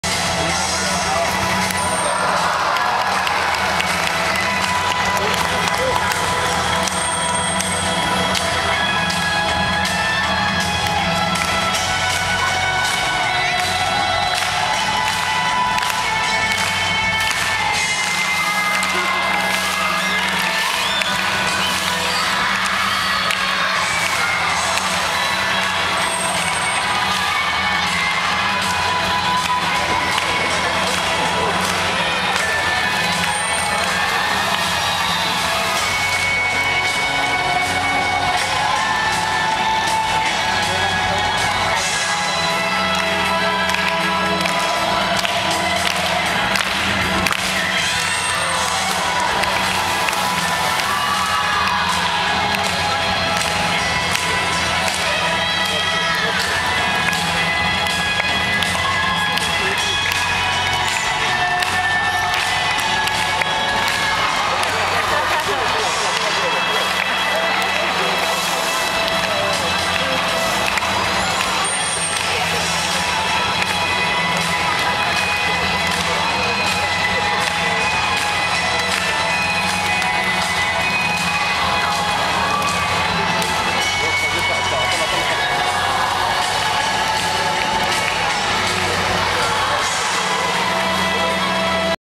Dolphin Show
Part of the dolphin show at the National Aquarium in Baltimore.